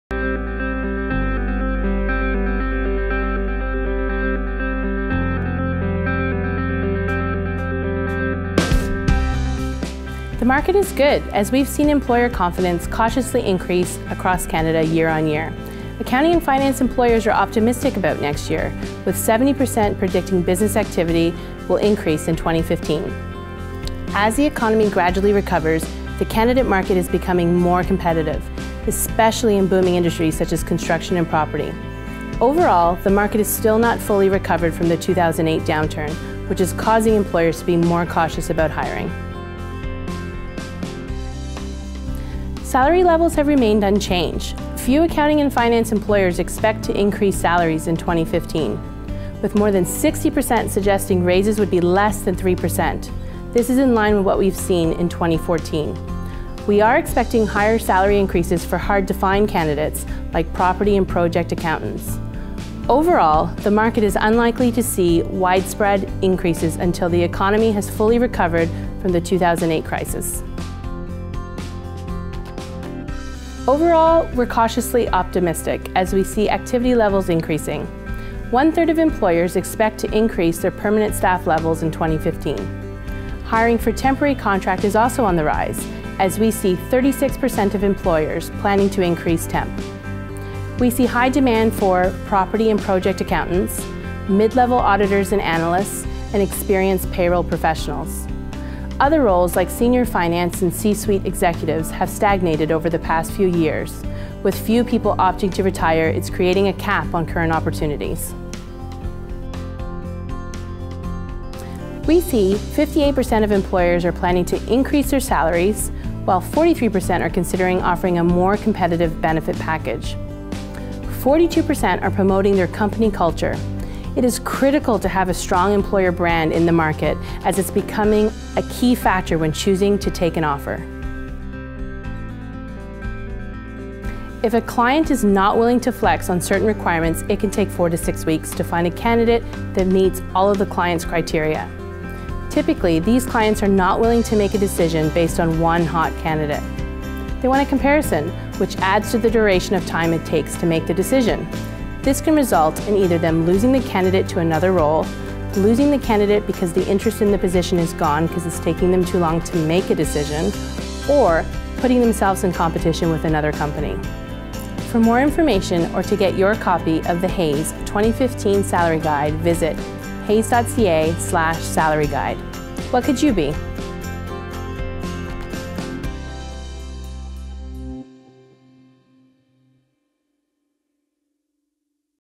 Accounting & Finance Labour Market - 2015
Get key insights into the accounting & finance sector as it relates to recruitment and retention, including compensation and benefit trends, hiring plans, and challenges that will face both employers and candidates.